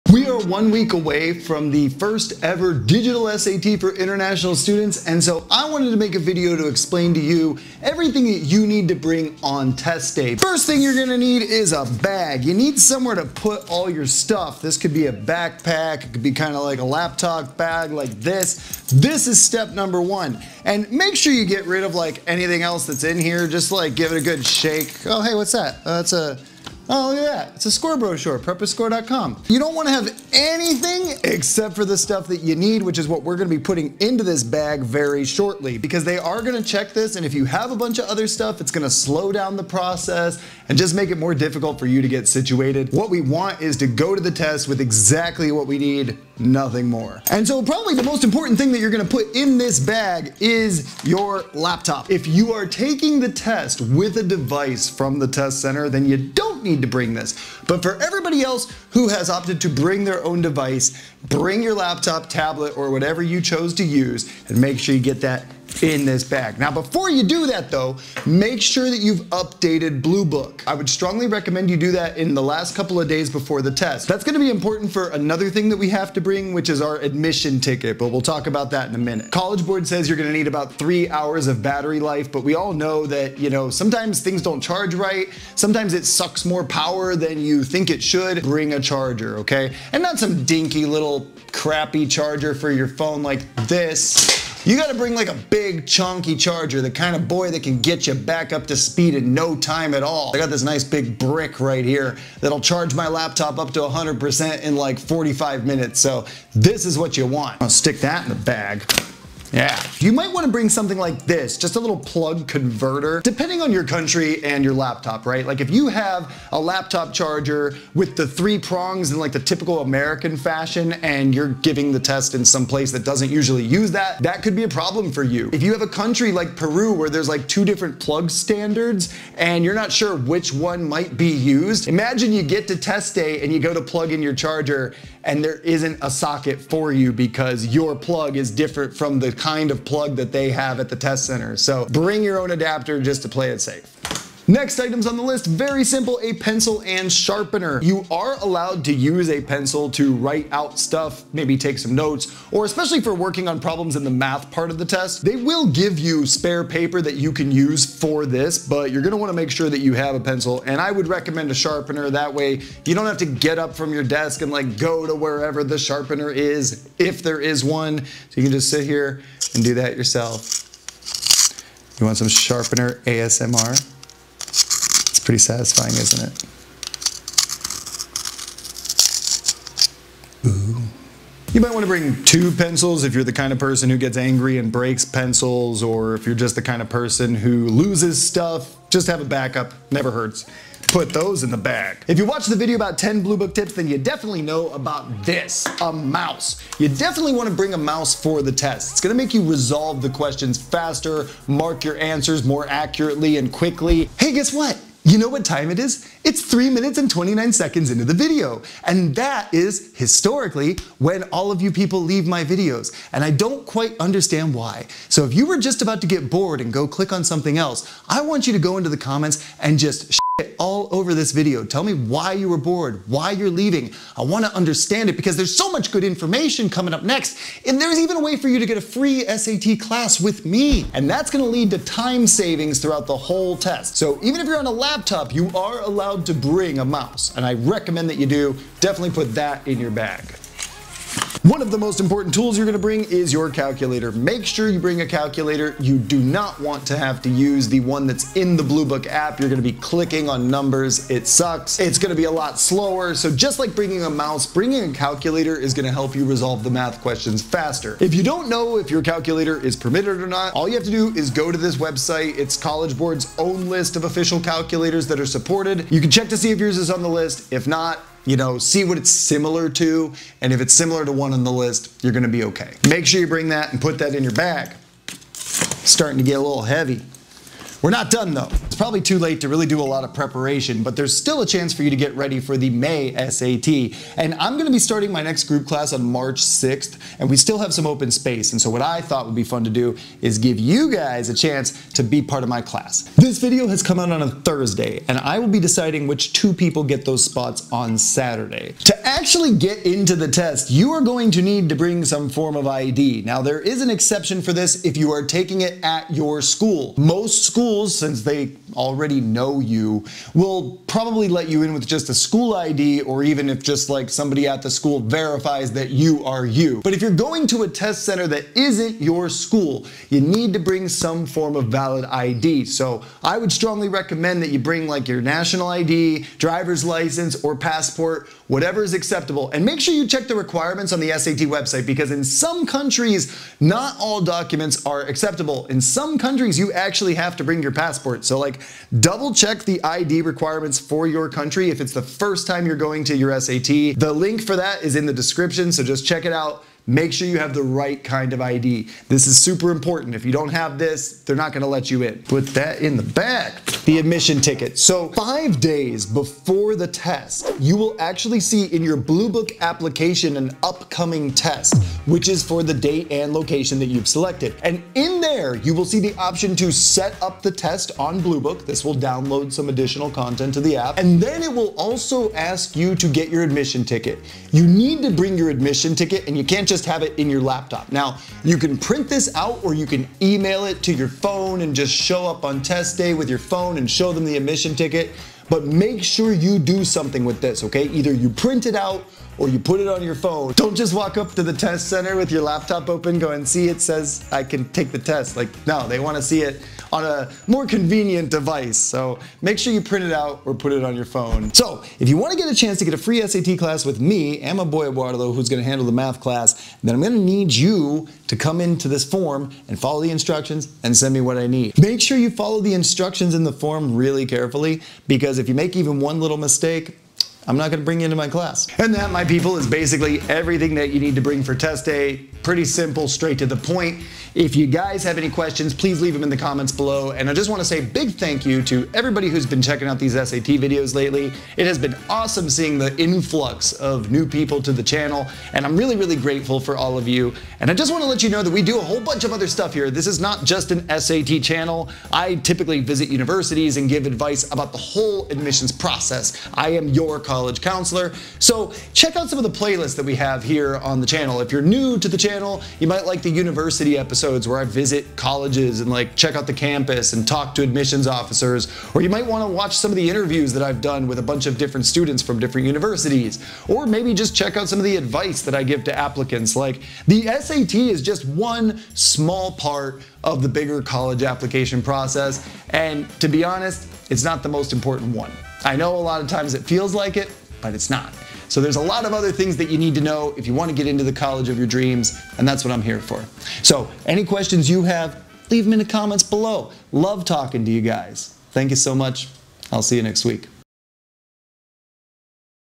What to Bring on Digital SAT Test Day
Pack your bag for the first ever Digital SAT test! Here's everything you need to bring to be ready for the March 11th Digital SAT Test Day!

Congrats to Mohammed and Sanjana for filling out the form correctly and getting a free spot in our May Digital SAT Group class! We'll be streaming the English classes here on Monday